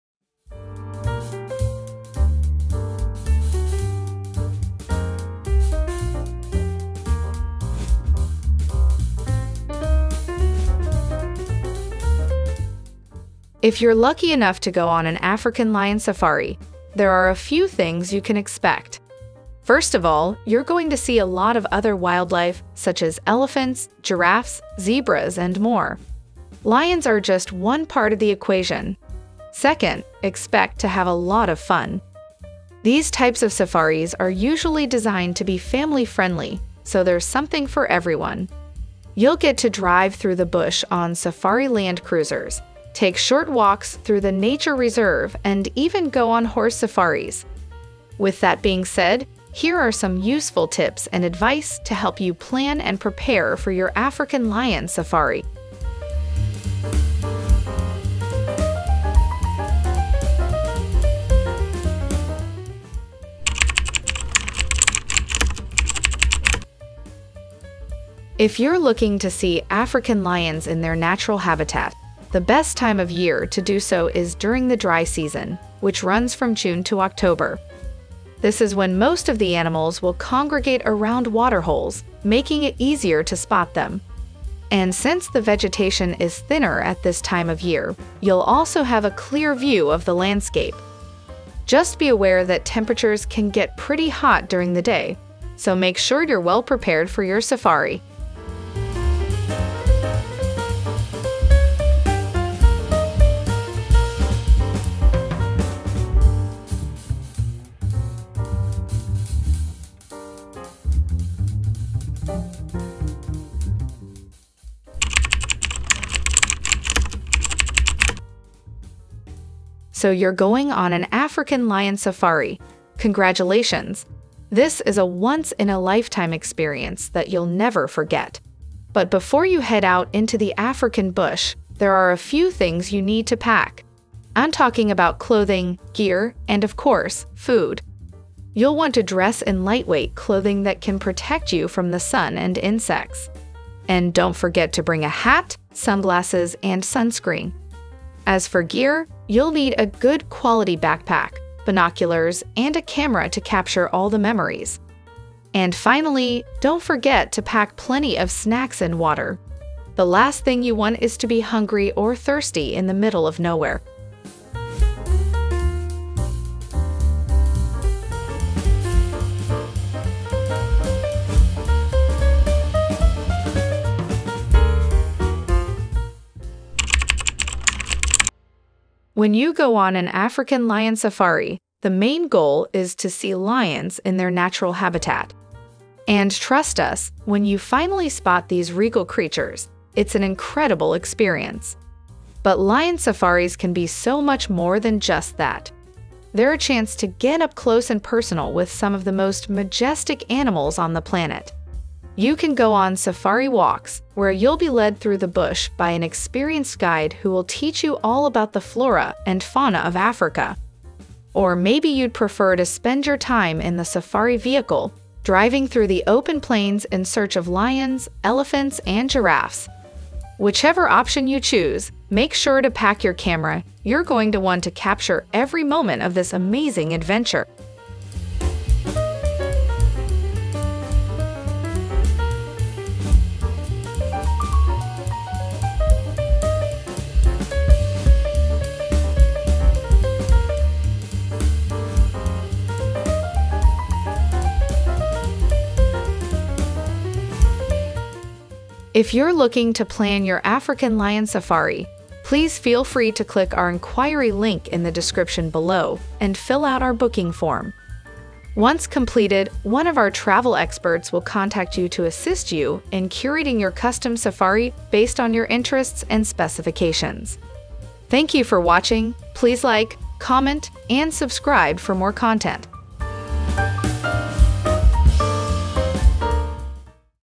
How to Prepare For Your African Lion Safari Trip - Travel Video
If you’re lucky enough to go on an African lion safari, there are a few things you can expect. First of all, you’re going to see a lot of other wildlife such as elephants, giraffes, zebras and more. Lions are just one part of the equation.

Second, expect to have a lot of fun. These types of safaris are usually designed to be family-friendly, so there’s something for everyone. You’ll get to drive through the bush on safari land cruisers, take walks through the nature reserve and even go on horse safaris.